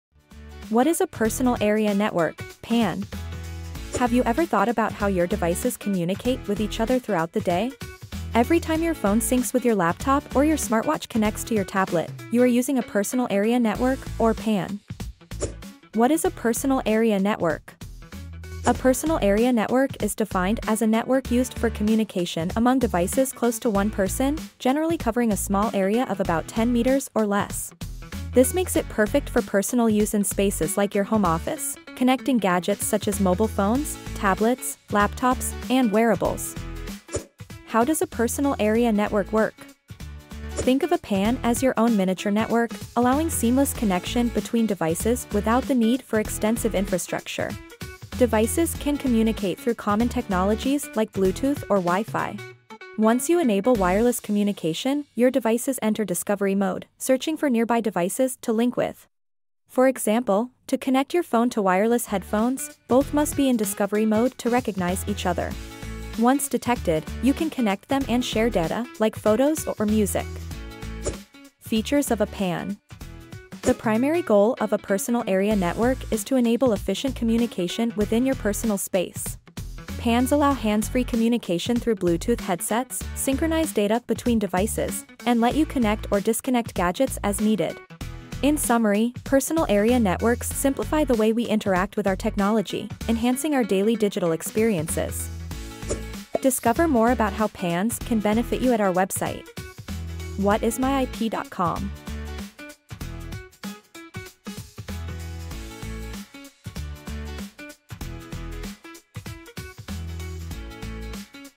What Is a Personal Area Network (PAN)?
🤔💻 Ever wondered how your gadgets talk to each other?

In this video, we're diving into the world of Personal Area Networks (PANs)!

✨ **What’s a PAN?** It’s a cozy little network that connects your devices—like your phone, laptop, and smartwatch—over a close range. Perfect for your home office setup!

✨ **How Does It Work?** Think of it as a mini network allowing your devices to chat seamlessly, thanks to technologies like Bluetooth and WiFi. Just turn on discovery mode, and watch them connect!

✨ **Key Features:** From hands-free calling with Bluetooth headsets to syncing data effortlessly, PANs make tech interactions super simple!

Want to learn more? Head to WhatIsMyIP.com!